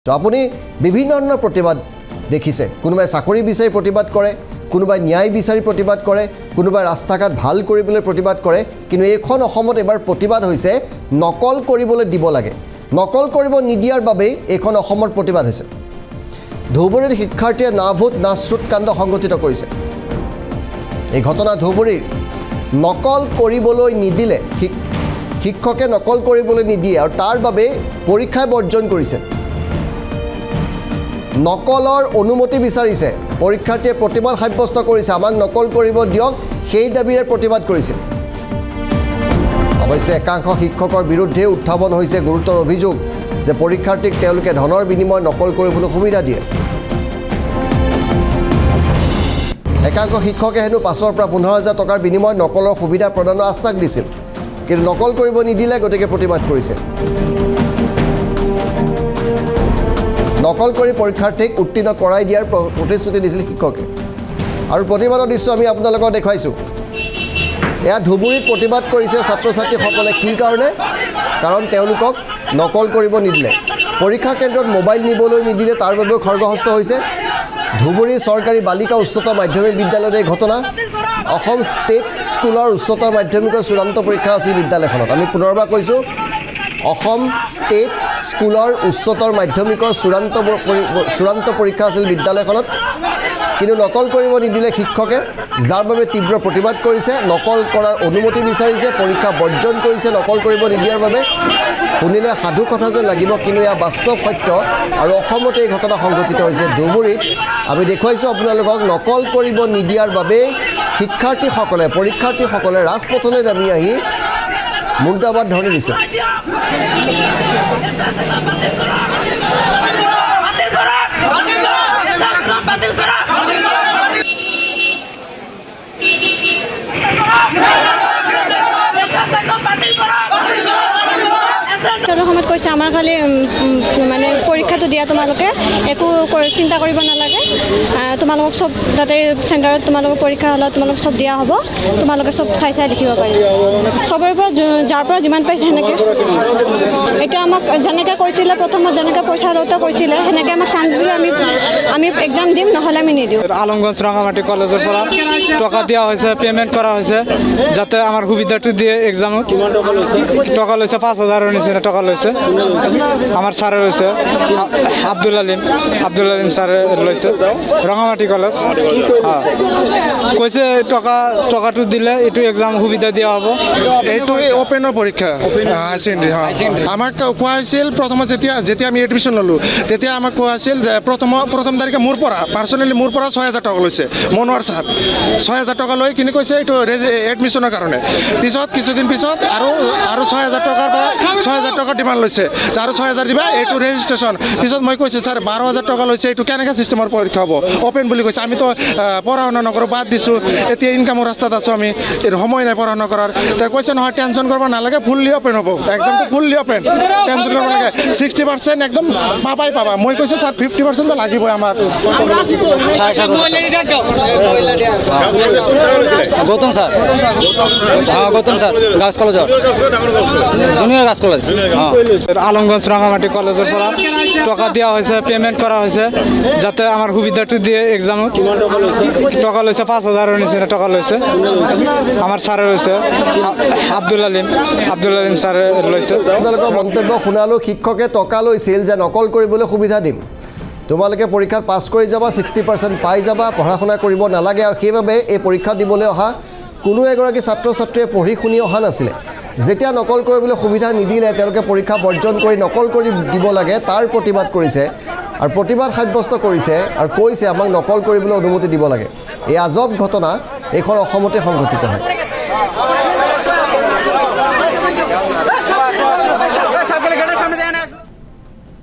ধুবুৰীত শিক্ষাৰ্থীৰ আচৰিত কাণ্ড। নকল কৰিব নিদিয়াৰ বাবেই পৰীক্ষাৰ্থীৰ উত্তাল প্ৰতিবাদ।
ধুবুৰীত শিক্ষাৰ্থীৰ আচৰিত কাণ্ড। নকল কৰিব নিদিয়াৰ বাবেই পৰীক্ষাৰ্থীৰ উত্তাল প্ৰতিবাদ। ধনৰ বিনিময়ত শিক্ষকেও শিক্ষাৰ্থীক নকল কৰিবলৈ দিয়াৰ অভিযোগ।